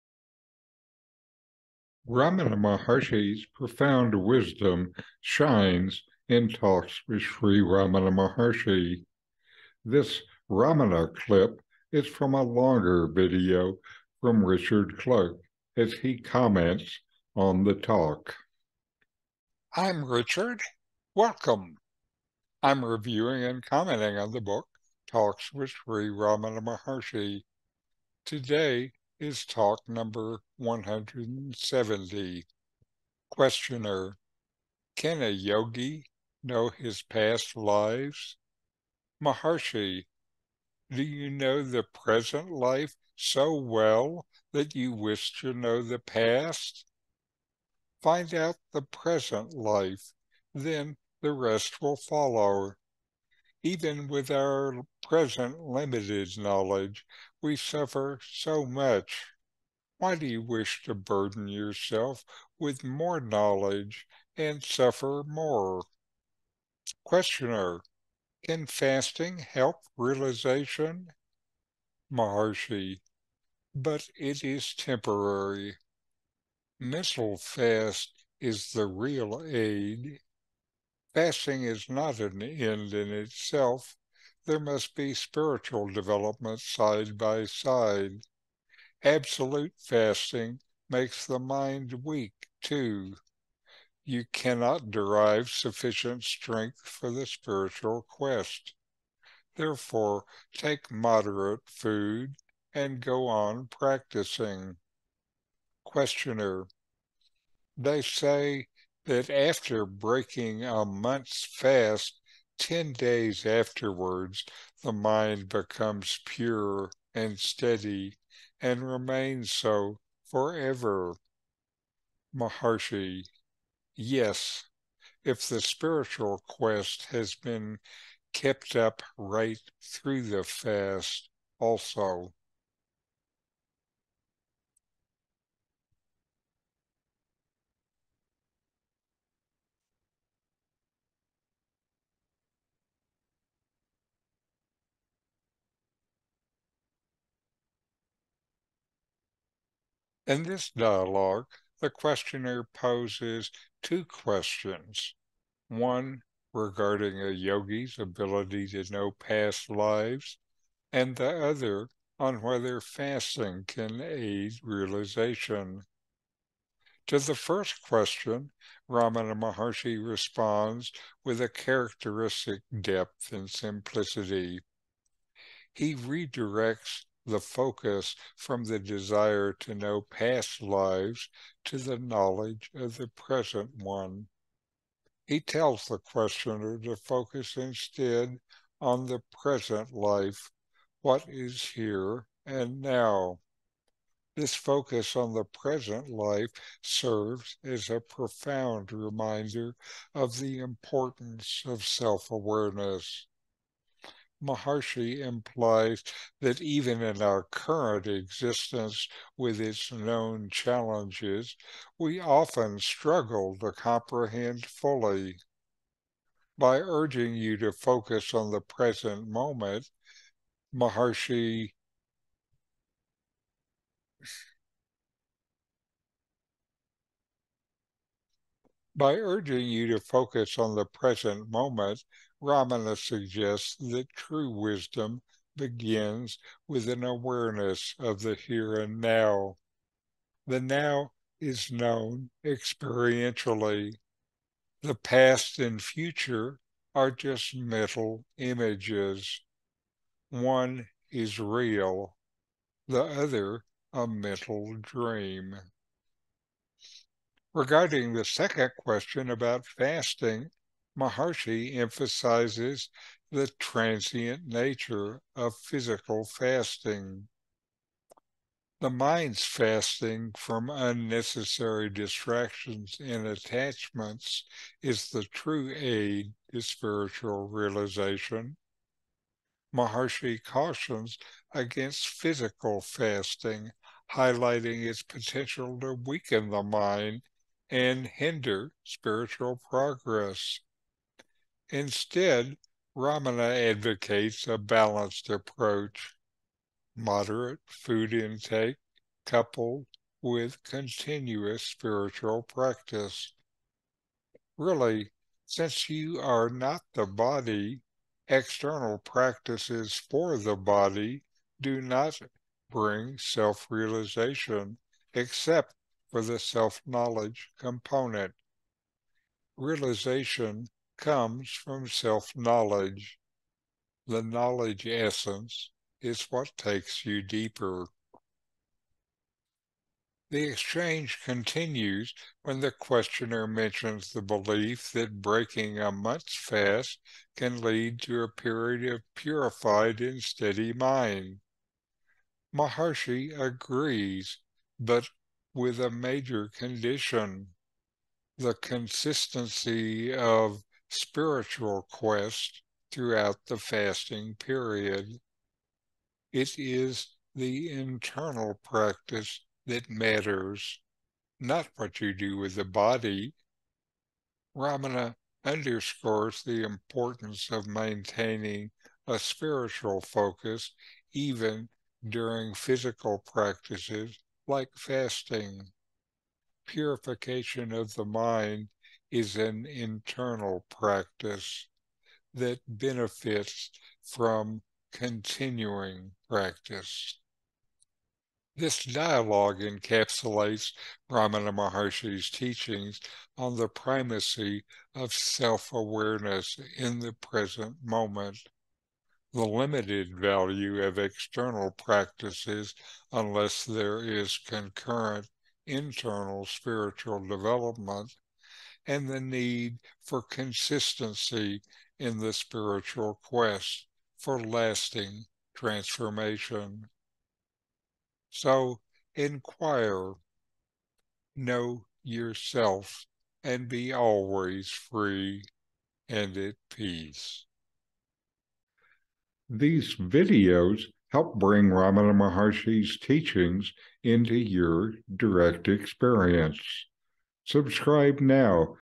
Keep up the spiritual quest - Ramana Clip Talk # 170
D.: Can fasting help realization?
Ramana Maharshi: But it is temporary. Mental fast is the real aid. Fasting is not an end in itself. There must be spiritual development side by side.

Comments by Richard Clarke. From "Talks  with Sri Ramana Maharshi."